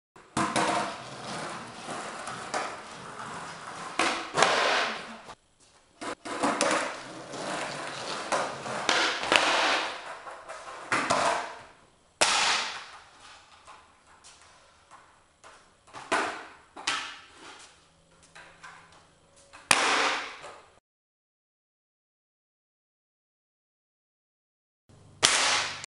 Double Flip = Snap Board
Yep, snapped my board, probably wont be skating for a while